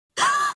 My Reaction That "El Deafo: The Movie" Has Not Confirmed A Release Date
Oh, it's terrible! "El Deafo: The Movie" has not confirmed a release date yet!